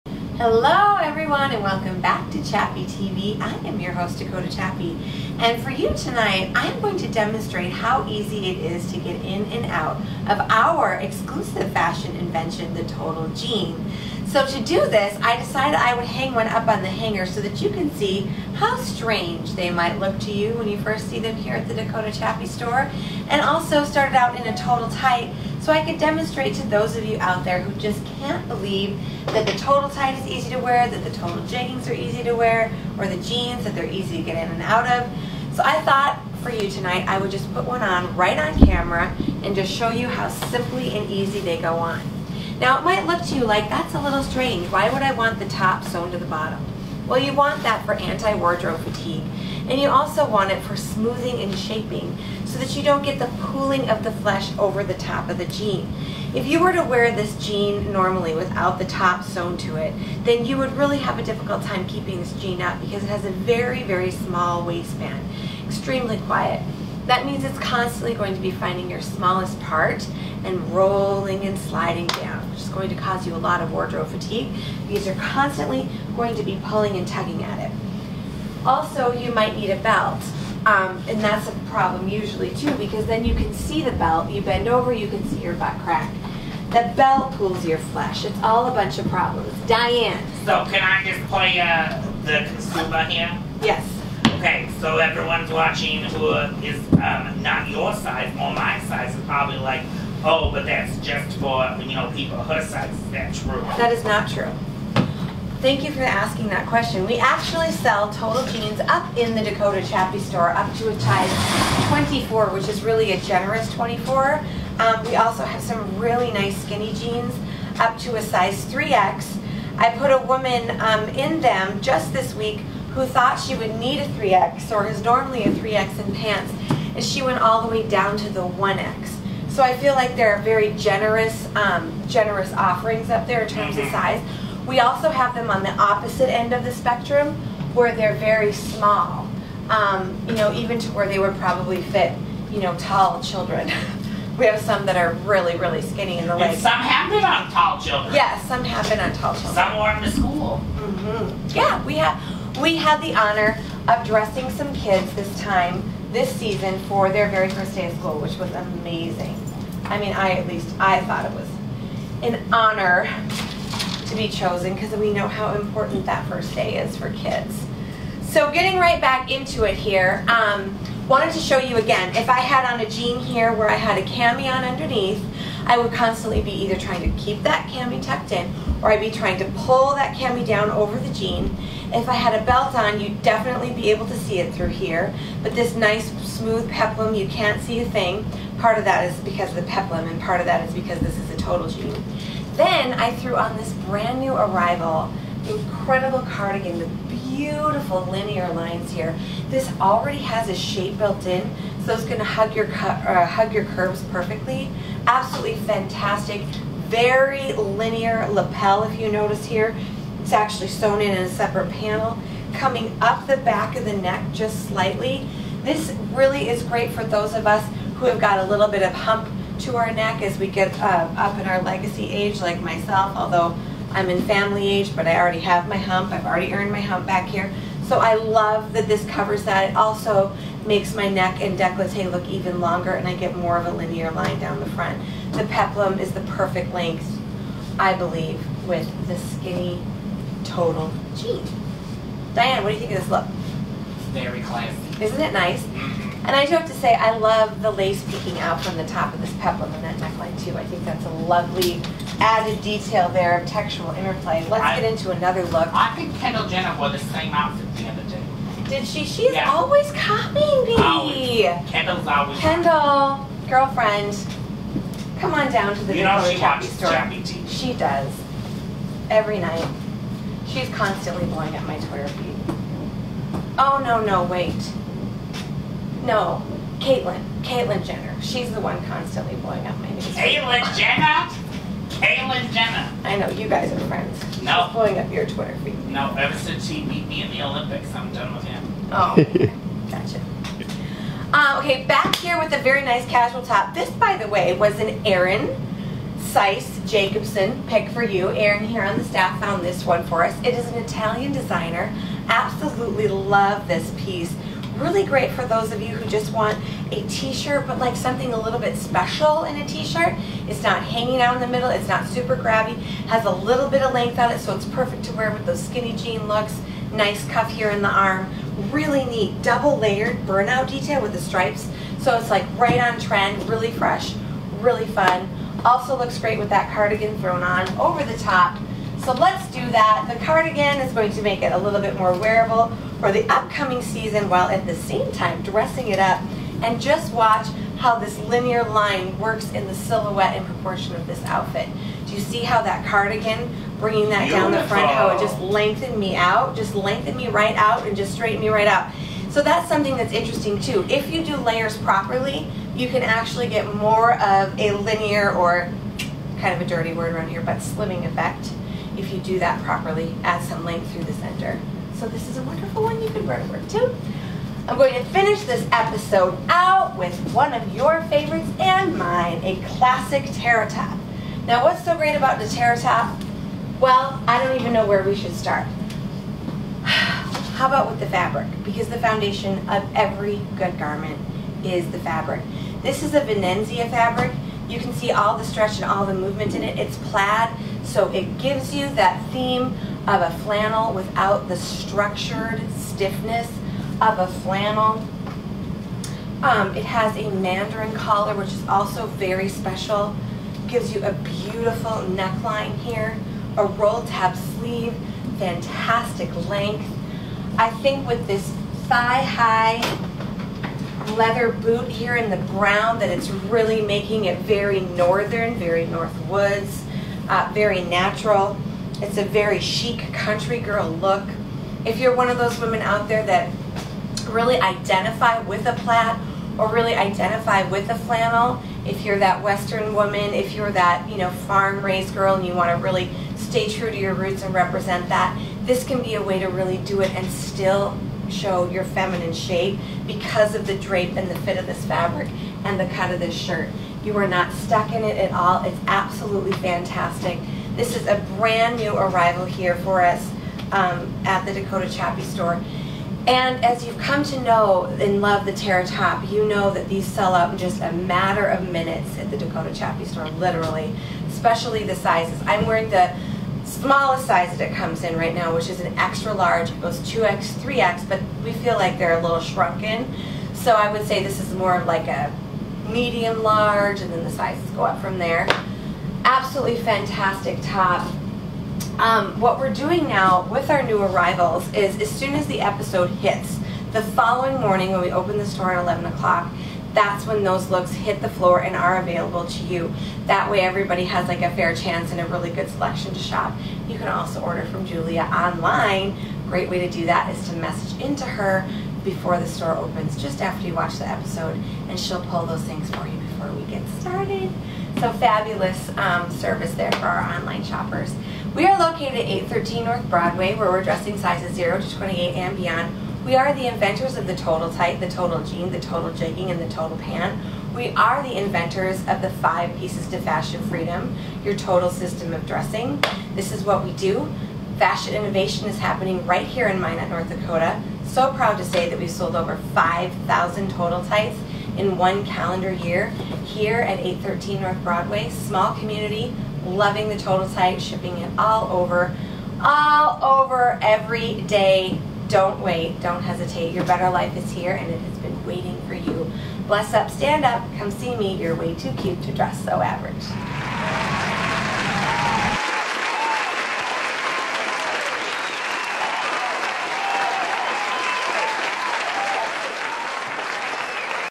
Episode #134
Tonight on Chappy TV
Total Jeans 101, We invented it, and now we want to answer your questions. How easy is it to get into the total jean? What is it all about? Watch this one! We show you how easy it really is and style up the perfect fall looks. Comment what you would wear to be entered to win a $25 store credit.